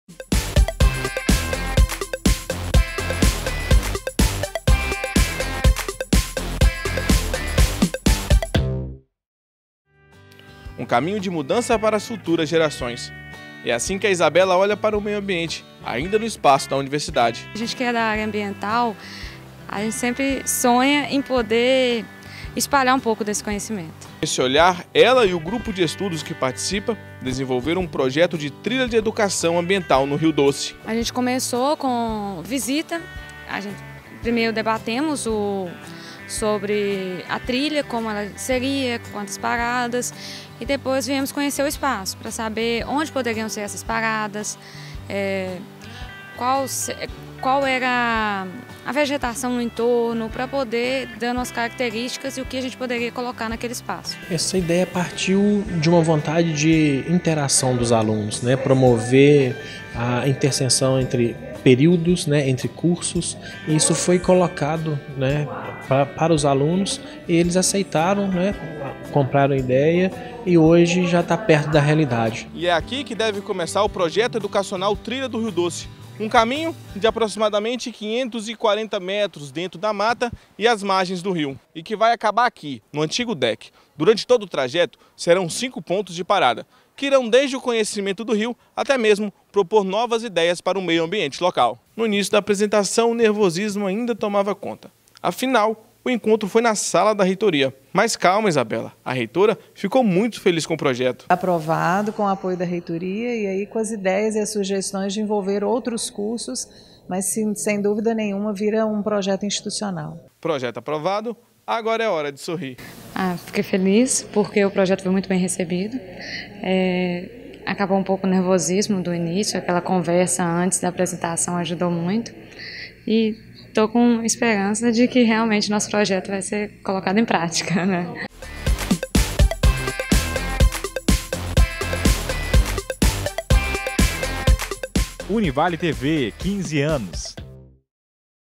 PROJETO TRILHA DE EDUCAÇÃO AMBIENTAL NA UNIVALE É APROVADO PELA REITORIA
Projeto voltado para educação ambiental no Rio Doce foi feito por um grupo de estudos e visa receber toda a comunidade. Confira mais essa historia.